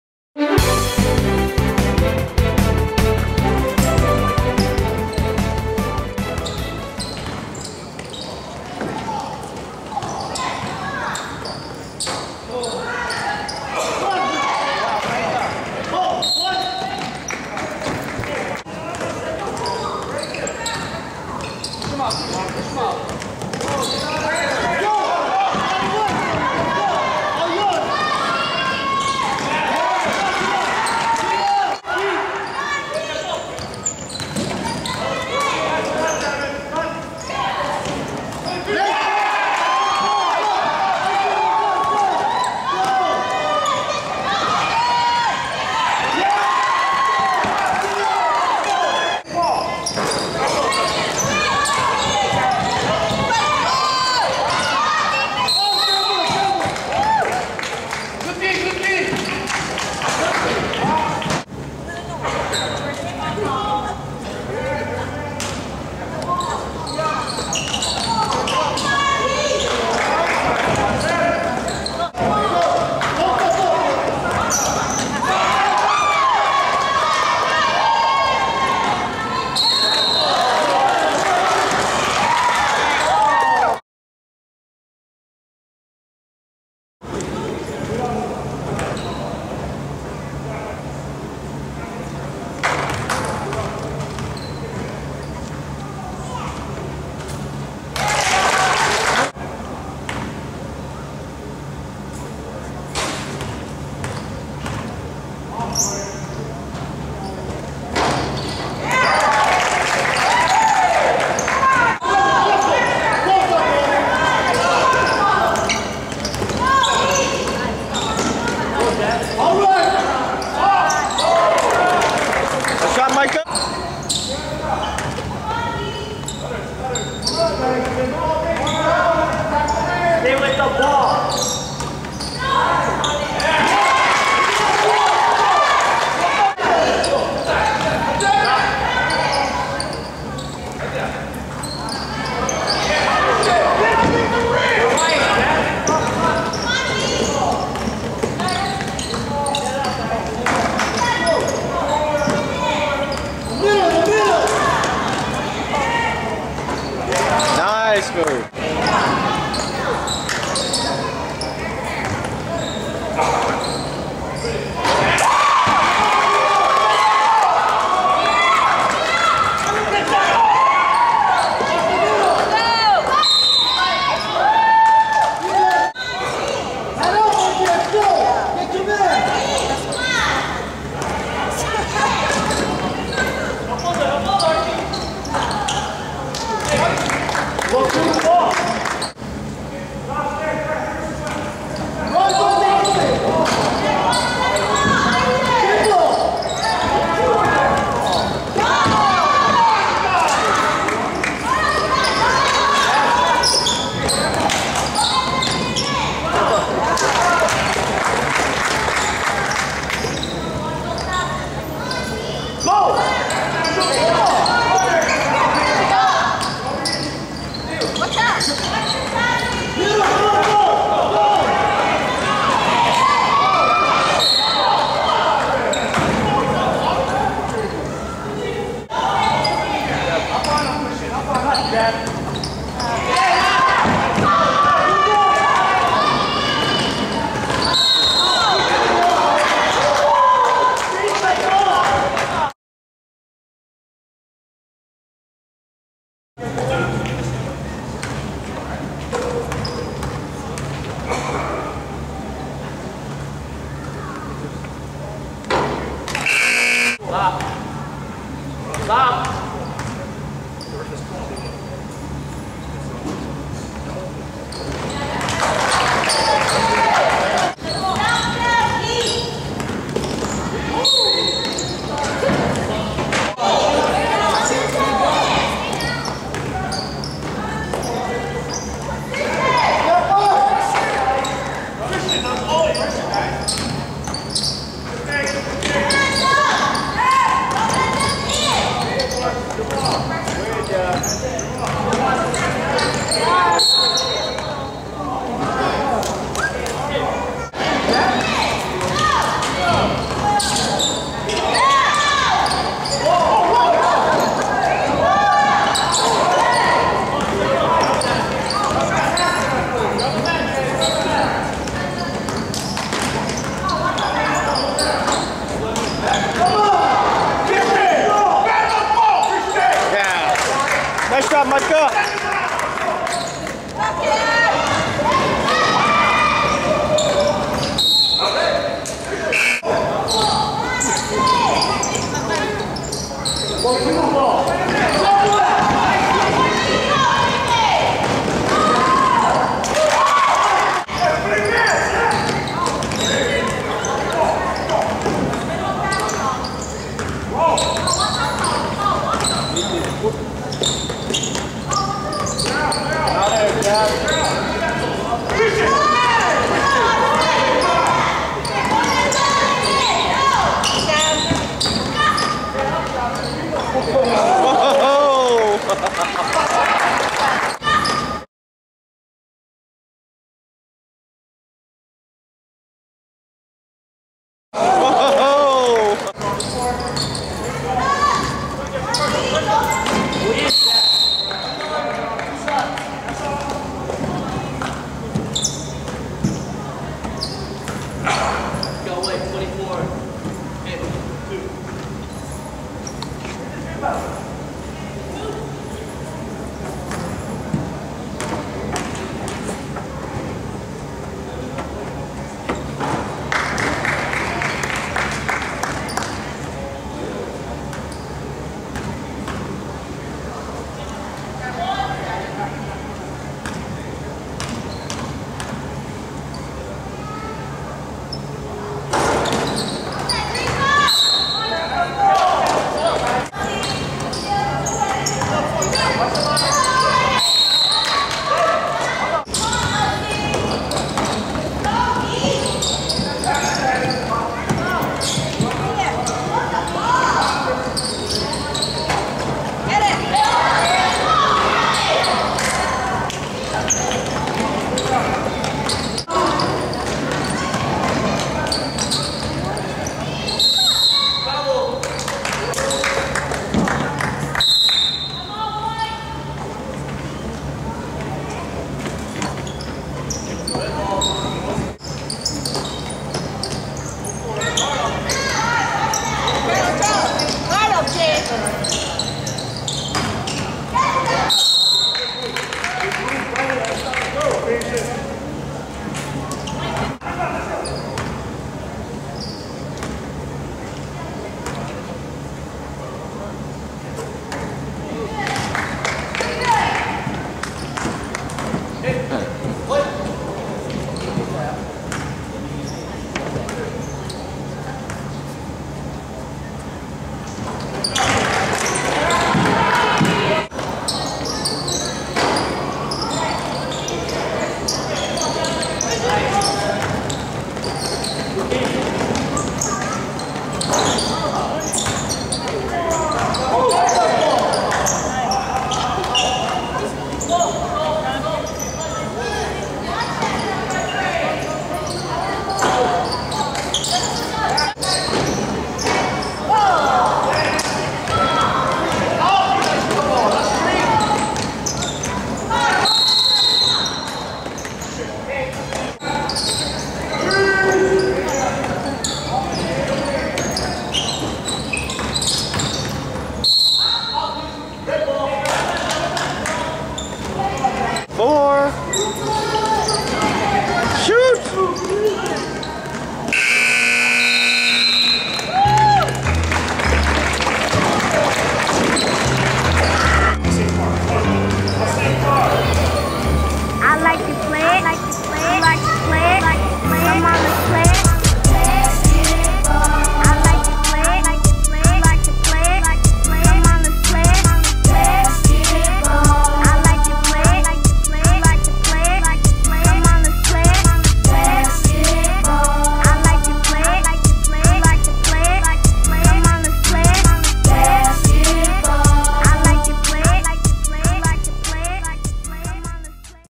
(2 of 2) H1 Thunder 12U vs Asing Park 5/18/2013
Part 2, H1 Thunder vs. Asing Park Heat, Aiea 12U League, Saturday, May 18, 2013, Aiea District Park Gym.